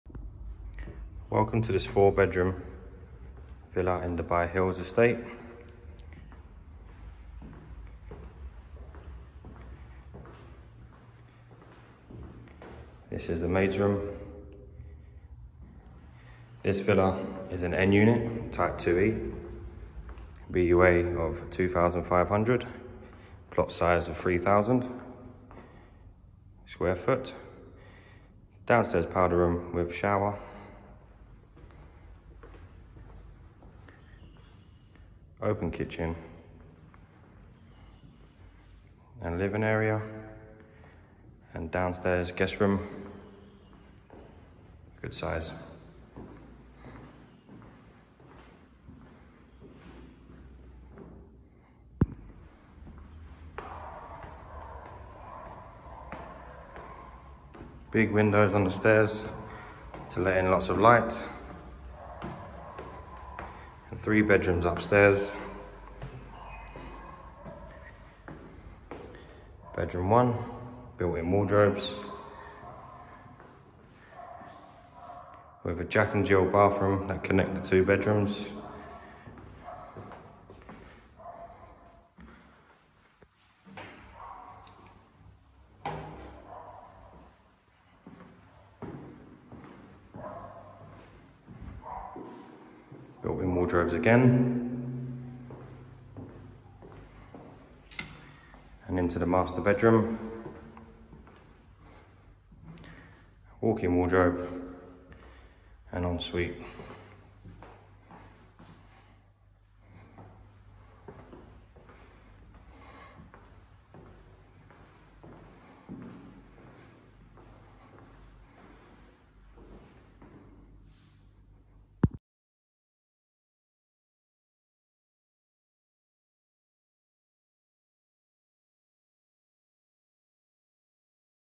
Ready to move now? Stunning 4-BR villa in Dubai Hills, vacant now!
Vacant Now | Single Row | Private

- Maple 1
- Type 2E
- Vacant on Transfer
- 4 Bedrooms plus Maid
- 3,108 Sq. Ft
- Single Row
- Private Location
- Ground Floor Bedroom
- Property Reference: PJM192480

Allsopp & Allsopp are delighted to offer this 4 bedroom townhouse in the maple 1 community of Dubai Hills Estate. On Entry, you will find a maid room, ground floor bedroom with downstairs shower room, living area, and doors to garden with fully fitted kitchen. First floor, 3 double bedrooms with en-suite, balcony.

Finance is available on this property through Allsopp & Allsopp Mortgage Services.

where you will find an extensive selection of properties available both for sale and for rent.